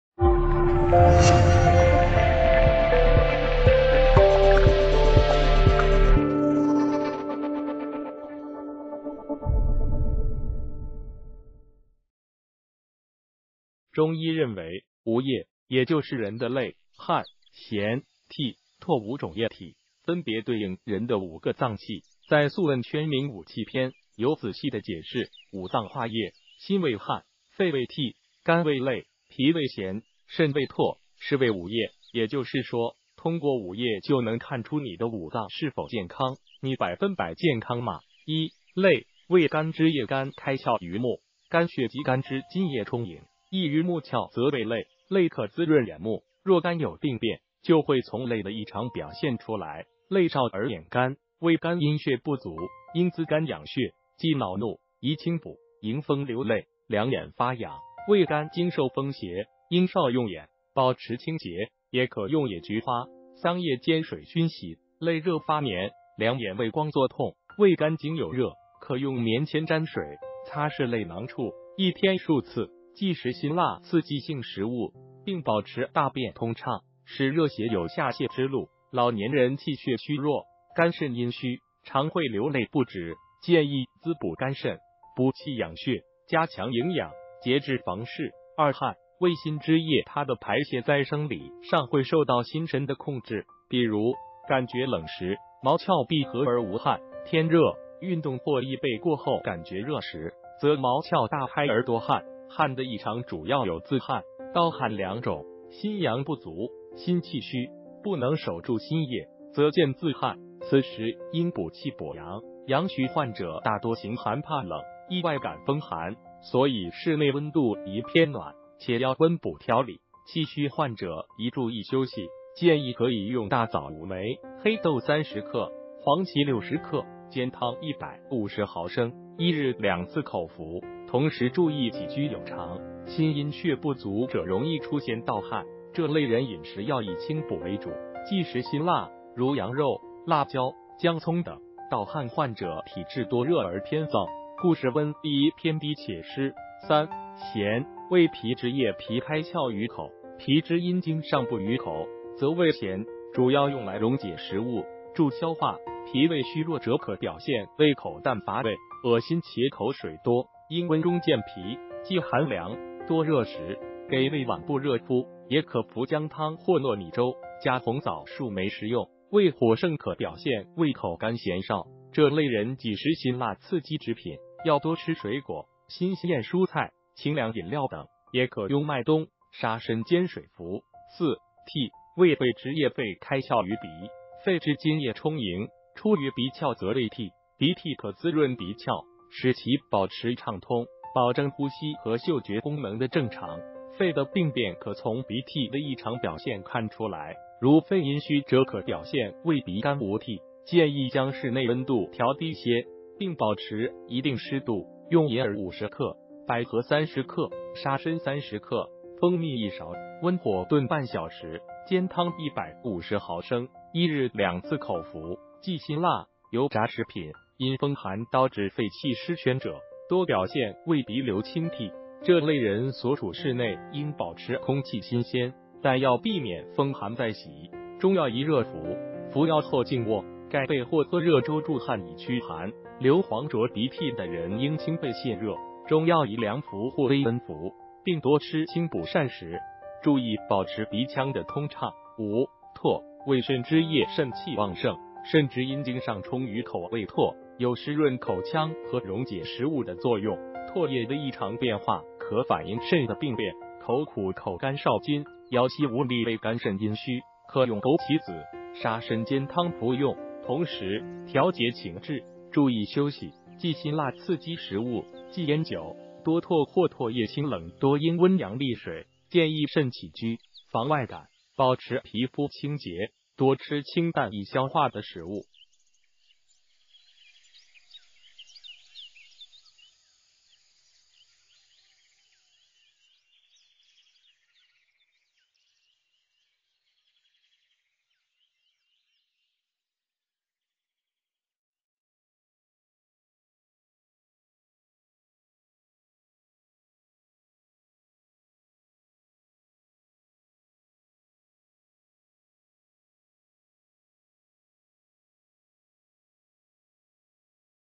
心为汗，肺为涕，肝为泪，脾为涎，肾为唾，从五液看五脏健康
谢谢！中医认为，五液，也就是人的泪、汗、涎、涕、唾五种液体，分别对应人的五个脏器，在《素问·宣明五气篇》有仔细的解释：“五脏化液，心为汗，肺为涕，肝为泪，脾为涎，肾为唾，是谓五液。”也就是说，通过五液就能看出你的五脏是否健康。你百分百健康吗？1、泪：为肝之液肝开窍于目，肝血及肝之津液充盈，溢于目窍则为泪。泪可滋润眼目，若肝有病变，就会从泪的异常表现出来。泪少而眼干，为肝阴血不足，应滋肝养血。忌恼怒，宜清补。迎风流泪、两眼发痒，为肝经受风邪，应少用眼，保持清洁，也可用野菊花、桑叶煎水熏洗。泪热发黏、两眼畏光作痛，为肝经有热，可用棉签沾水、擦拭泪囊处，一天数次。忌食辛辣刺激性食物，并保持大便通畅，使热邪有下泻之路。老年人气血虚弱、肝肾阴虚，常会流泪不止。建议滋补肝肾，补气养血，加强营养，节制房事。2、汗：为心之液它的排泄在生理上会受到心神的控制，比如，感觉冷时，毛窍闭合而无汗;天热、运动或衣被过厚感觉热时，则毛窍大开而多汗。汗的异常主要有自汗、盗汗两种。心阳不足、心气虚，不能“守住”心液，则见自汗，此时应补气补阳。阳虚患者大多形寒怕冷，易外感风寒，所以室内温度宜偏暖，且要温补调理。气虚患者宜注意休息，建议可以用大枣5枚、黑豆30克、黄芪60克、煎汤150毫升，一日两次口服，同时注意起居有常。心阴血不足者容易出现盗汗，这类人饮食要以清补为主，忌食辛辣，如羊肉、辣椒、姜葱等。盗汗患者体质多热而偏燥，故室温宜偏低且湿。3、涎：为脾之液脾开窍于口，脾之阴精上布于口，则为涎，主要用来溶解食物，助消化。脾胃虚弱者可表现为口淡乏味，恶心且口水多，应温中健脾。忌寒凉、多热食，给胃脘部热敷，也可服姜汤或糯米粥加红枣数枚食用。胃火盛可表现为口干涎少，这类人忌食辛辣刺激之品，要多吃水果、新鲜蔬菜、清凉饮料等，也可用麦冬、沙参煎水服。4、涕：为肺之液肺开窍于鼻，肺之津液充盈，出于鼻窍则为涕。鼻涕可滋润鼻窍，使其保持畅通，保证呼吸和嗅觉功能的正常。肺的病变可从鼻涕的异常表现看出来。如肺阴虚者可表现为鼻干无涕，建议将室内温度调低些，并保持一定湿度。用银耳50克、百合30克、沙参30克、蜂蜜一勺，温火炖半小时，煎汤150毫升，一日两次口服。忌辛辣、油炸食品。因风寒导致肺气失宣者，多表现为鼻流清涕。这类人所处室内应保持空气新鲜，但要避免风寒再袭。中药宜热服，服药后静卧，盖被或喝热粥助汗以驱寒。流黄浊鼻涕的人应清肺泄热。中药宜凉服或微温服，并多吃清补膳食，注意保持鼻腔的通畅。5、唾：为肾之液肾气旺盛，肾之阴精上充于口为唾，有湿润口腔和溶解食物的作用。唾液的异常变化可反映肾的病变。口苦口干少津，腰膝无力为肝肾阴虚，可用枸杞子、沙参煎汤服用。同时，调节情志，注意休息，忌辛辣刺激食物，忌烟酒。多唾或唾液清冷多应温阳利水。建议慎起居、防外感，保持皮肤清洁，多吃清淡易消化的食物。